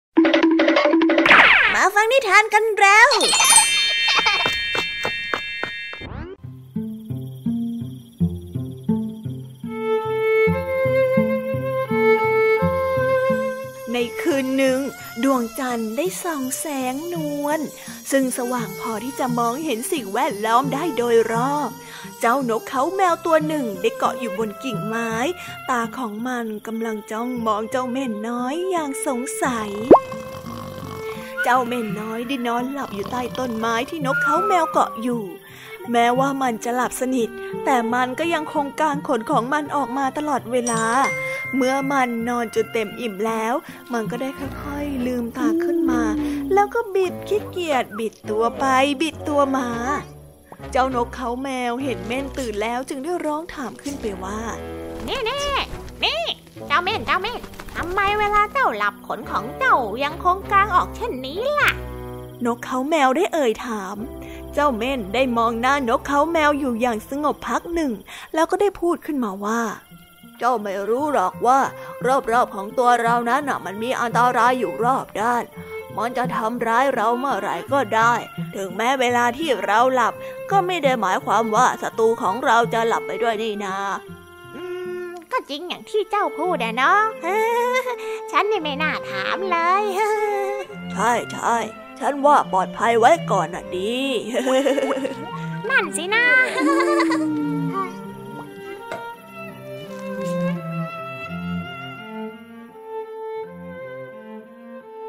นิทาน ระแวดระวังภัย | Kids Hour
เมื่อนกเค้าแมวไม่เข้าใจ ว่าทำไมเจ้าเม่นจึงต้องกางขนอยู่ตลอดเวลา แม้กระทั่งในตอนที่หลับอยู่ นั่นจึงทำให้เจ้าเม่นต้องอธิบายในสิ่งที่มันทำ เพื่อให้นกเค้าแมวหายสงสัย ไปฟังเรื่องราวนี้พร้อมกันได้เลย